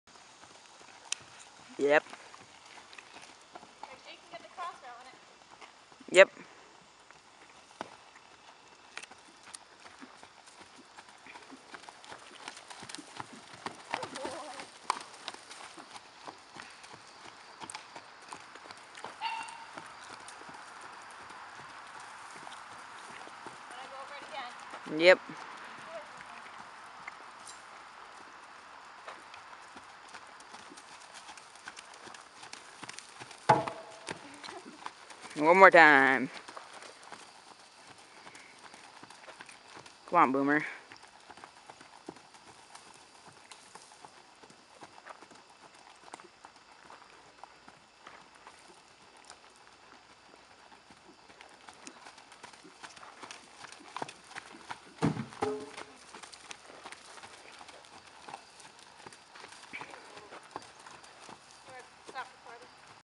Boomer cross rails August 2013
Training my OTTB to jump